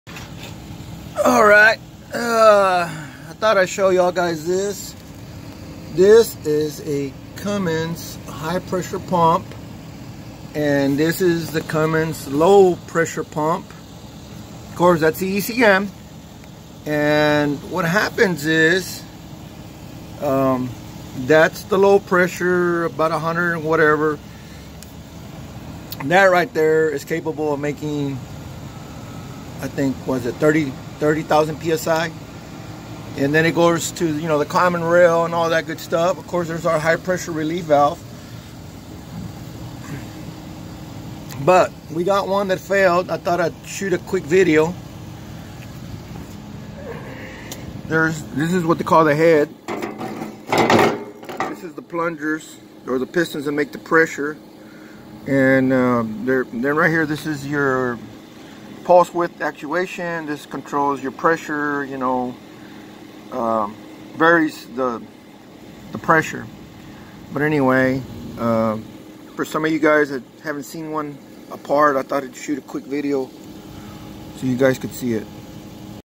Cummins ISX Fuel pump fail!!
Quick view of a hi pressure fuel pump